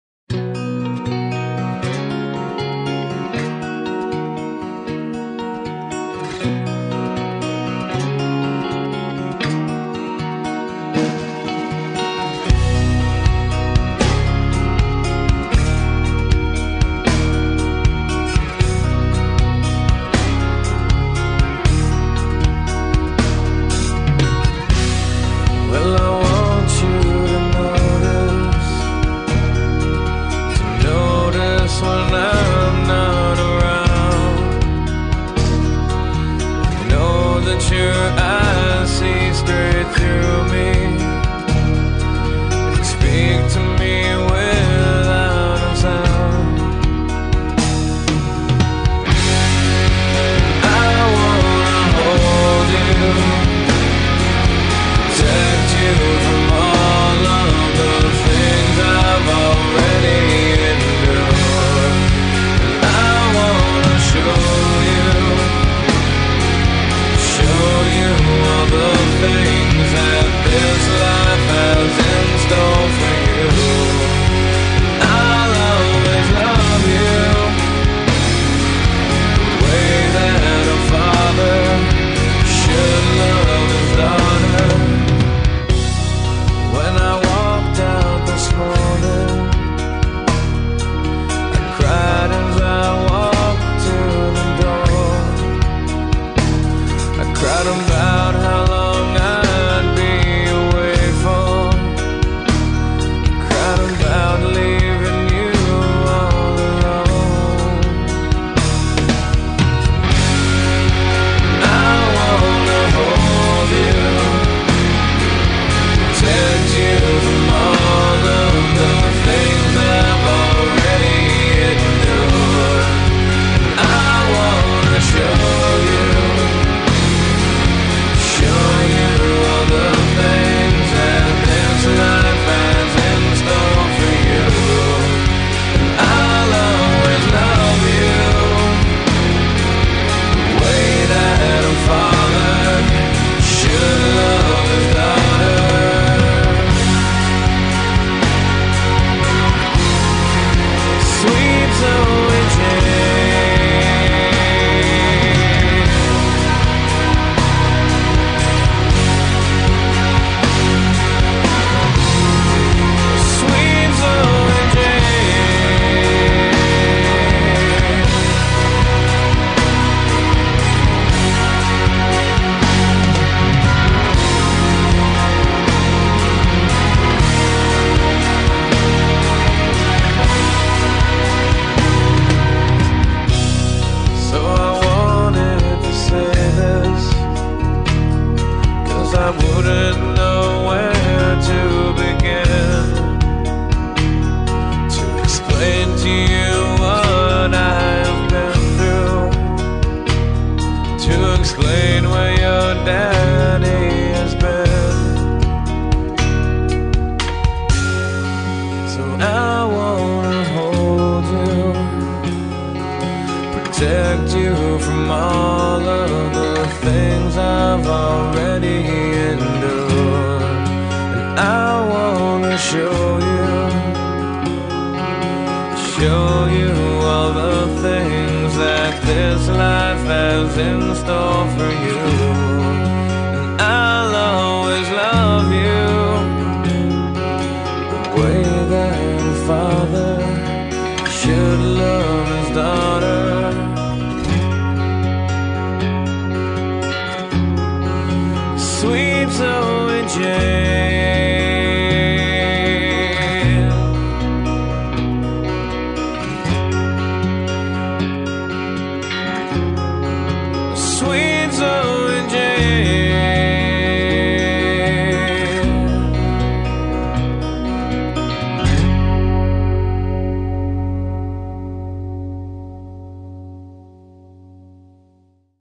Welcome to this world Zoe Jane Glover
This is a slideshow that I made for Zoe's first days on earth.